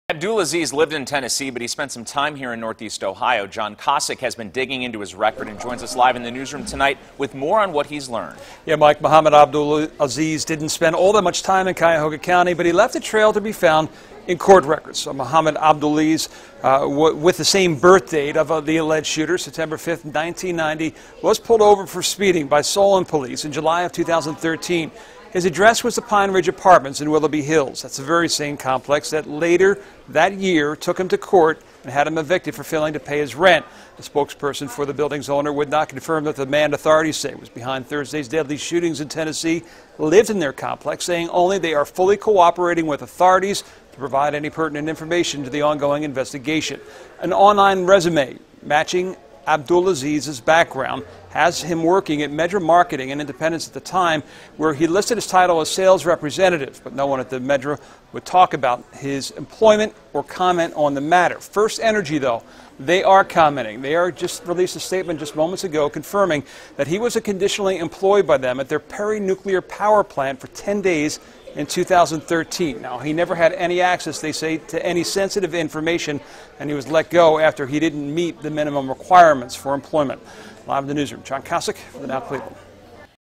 Chattanooga gunman Mohammad Youssef Abdulazeez lived, worked in Northeast Ohio
The man who opened fire at two military offices in Tennessee worked at the Perry Nuclear Plant in 2013, FirstEnergy reports.